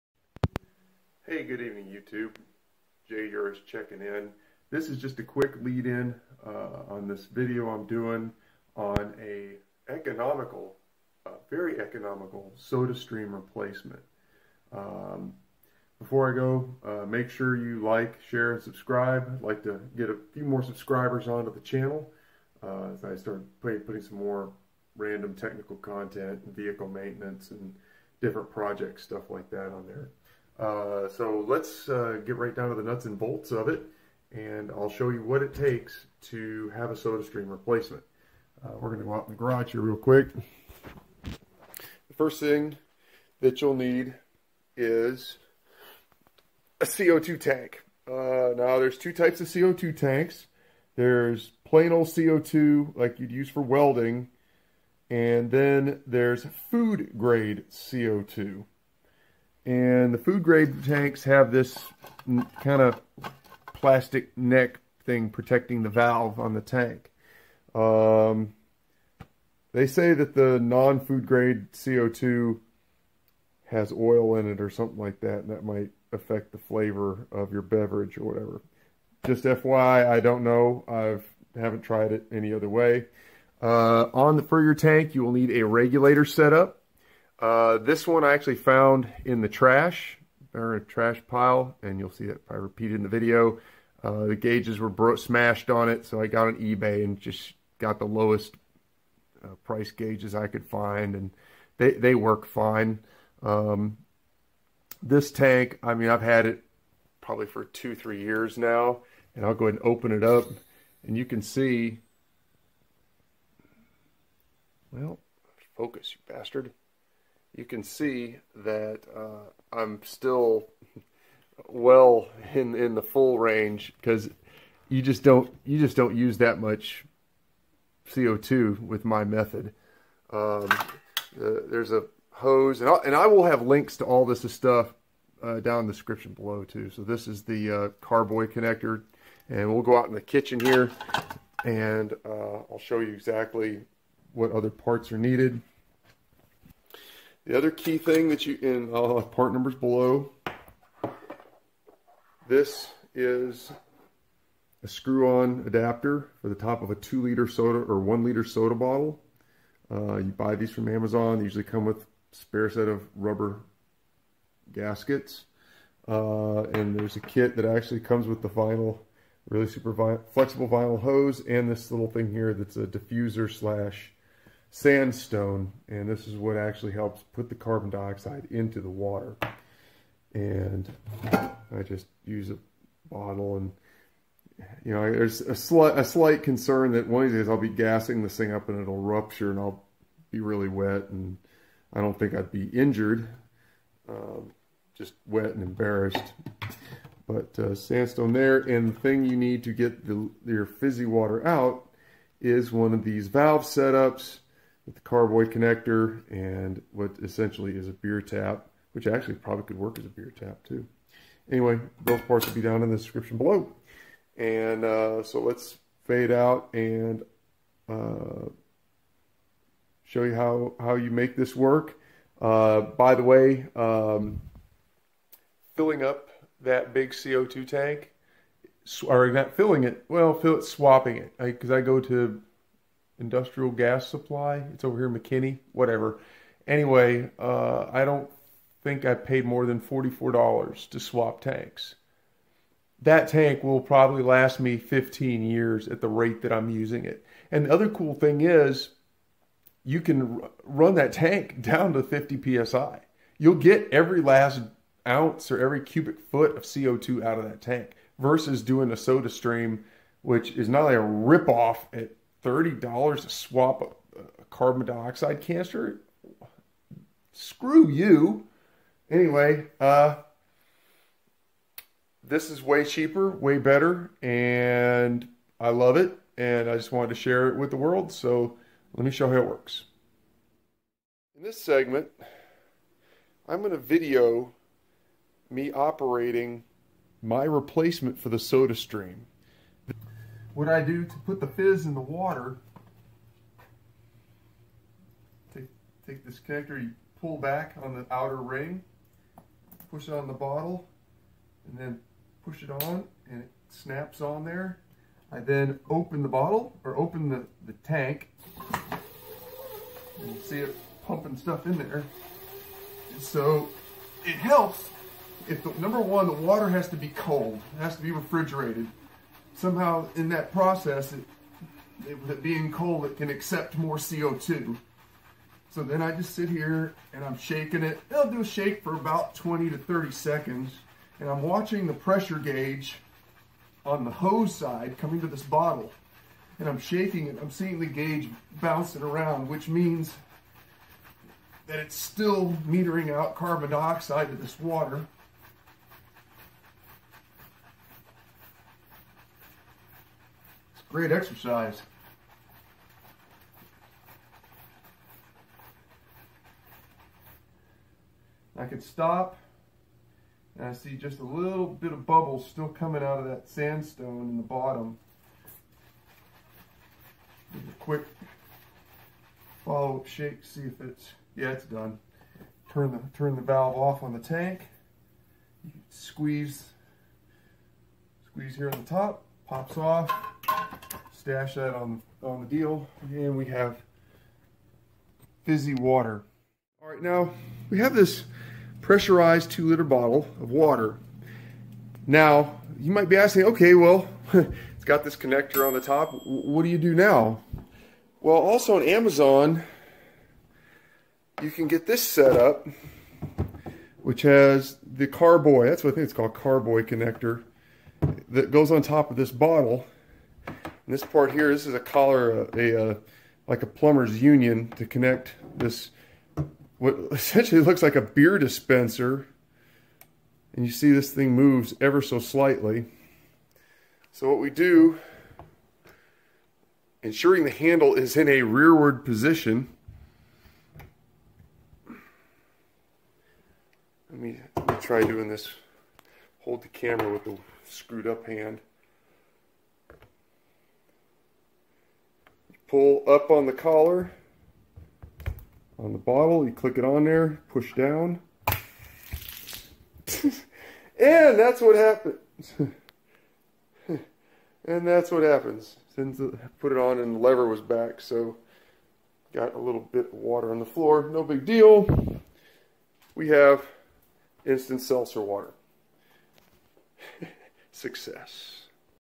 A home built Soda Stream alternative! Cheaper seltzer water!
This is my Soda Stream replacement setup.

This is the parts list.

MRbrew Carbonation Cap 304 Stainless Steel with 2 Micron Diffusion Stone and 12'' Silicone Tube(Cap with Stone)

Stainless Steel Stem Beer keg Tap Faucet with Ball Lock disconnect chromed body
by TOMMUR

The Weekend Brewer Ball Lock Cornelius Corny Keg Gas Line Assembly
by HomeBrewStuff

*I am NOT using this regulator*
However, it should work ok or something similar.

SKYTOU Dual Gauge CO2 Regulator, Keg Beer Regulator CGA-320 Inlet for Draft Beer Homebrew Kegerator Dispensing Regulator
by SUYOO